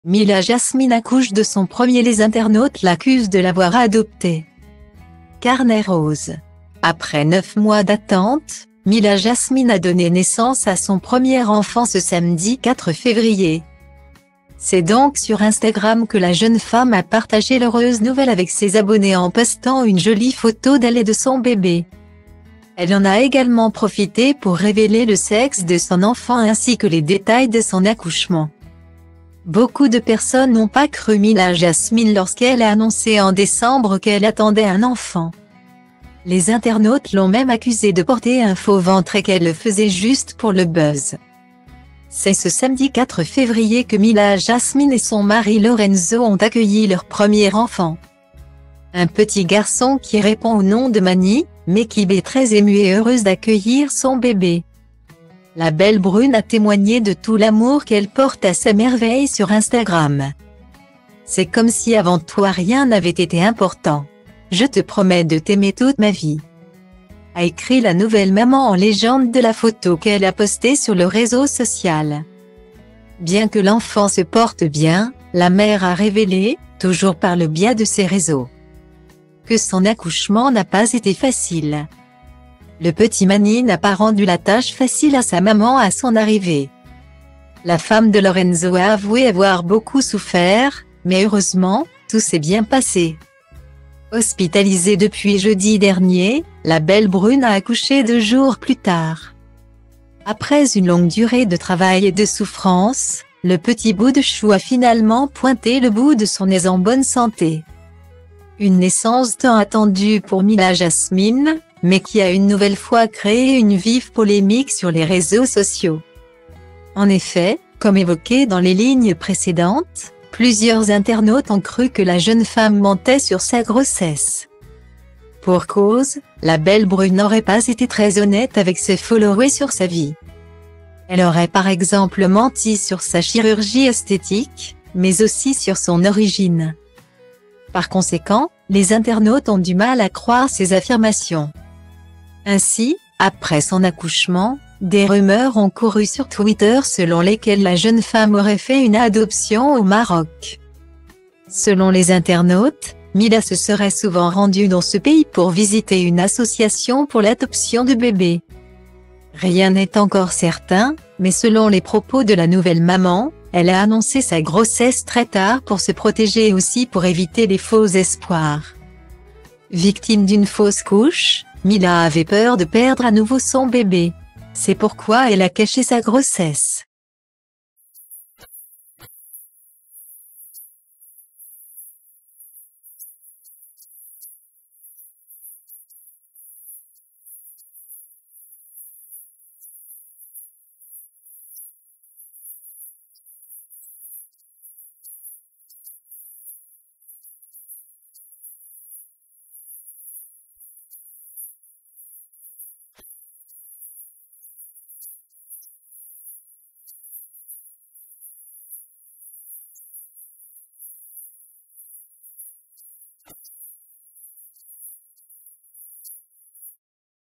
Milla Jasmine accouche de son premier les internautes l'accusent de l'avoir adopté
Carnet rose ! Après 9 mois d'attente, Milla Jasmine a donné naissance à son premier enfant ce samedi 4 février.C'est donc sur Instagram que la jeune femme a partagé l'heureuse nouvelle avec ses abonnés en postant une jolie photo d'elle et de son bébé.Elle en a également profité pour révéler le sexe de son enfant ainsi que les détails de son accouchement.